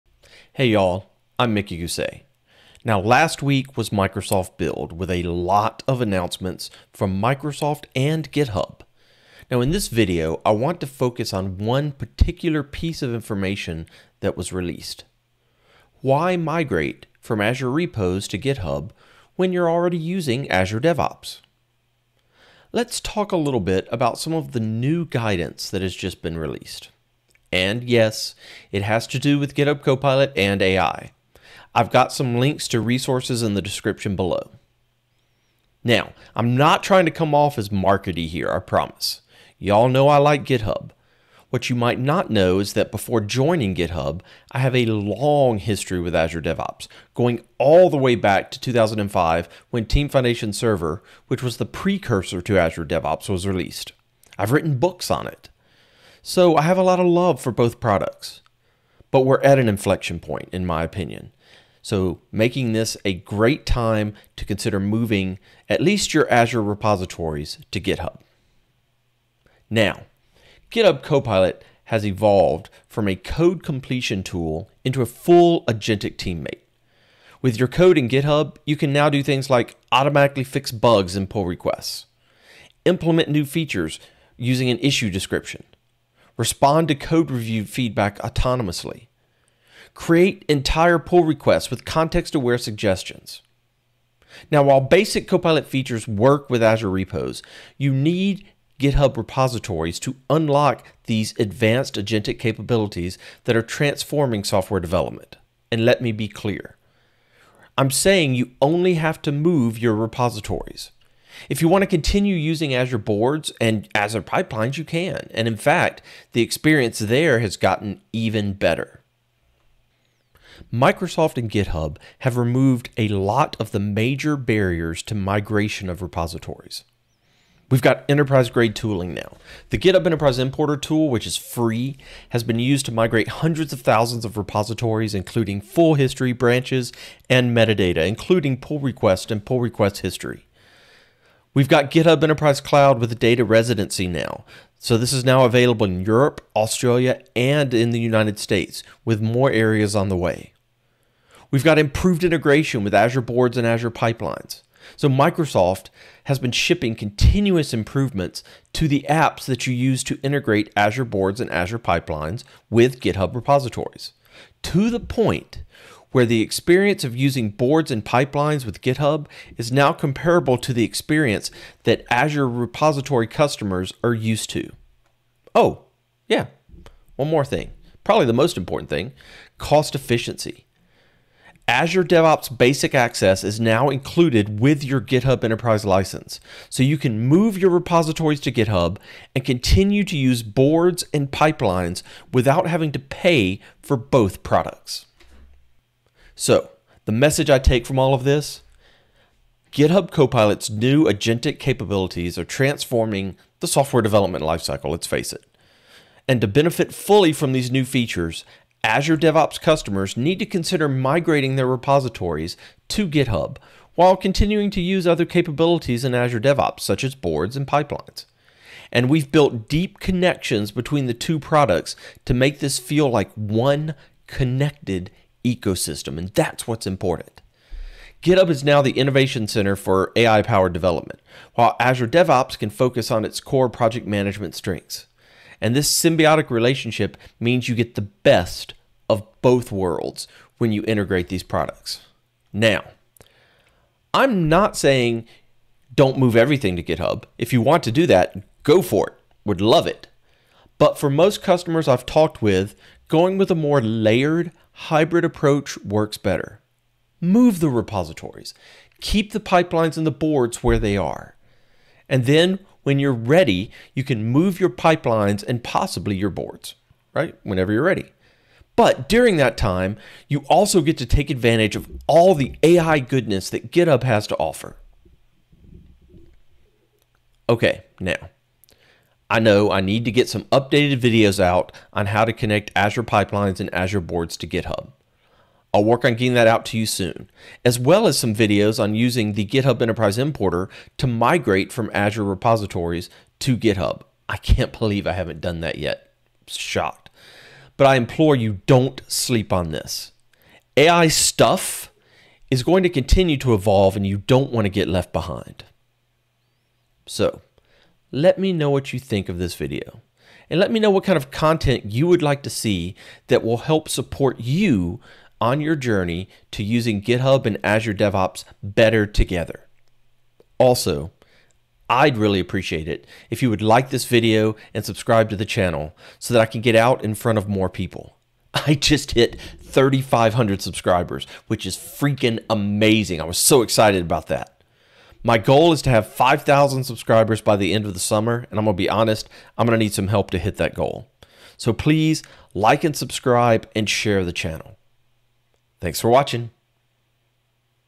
Azure DevOps with GitHub Repositories – Your path to Agentic AI
Copilot has transformed into an intelligent development partner that can review your pull requests, automatically fix bugs, implement new features, create PRs, and handle feedback responses. These game-changing capabilities are already reshaping the entire software development process for teams at Microsoft and GitHub.

To unlock Copilot's full potential, your code needs to be on GitHub. If you're currently using Azure DevOps repositories, now is the perfect time to migrate to GitHub and experience these powerful agentic features while keeping all your existing Azure Boards and Pipelines investments.

This short video is going to explain to you "why" you should move your Azure Repos to GitHub, and set up the integration between Azure Pipelines and Azure Boards to GitHub.

I've also got future videos coming that show you the technical portion of how to do all of this.